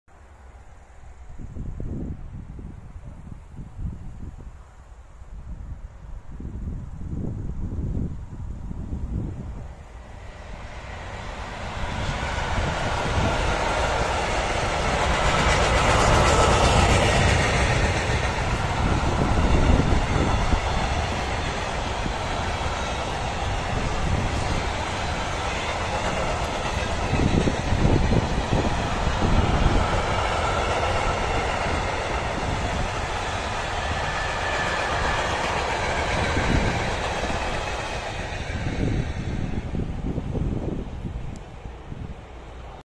E6ACTa-016 "Usia" Cargounit z węglarkami | Świebodzin
Po 2 latach ponownie złapałem Dragona II w barwach Cargounit, o imieniu Usia. Maszyna od nowości jeździ w ČD Cargo Poland.

Film

E6ACTa-016 z węglarkami w planie 484015 wjeżdża do Świebodzina od strony Zbąszynka
Relacja składu
Chałupki - Świnoujście Port SIA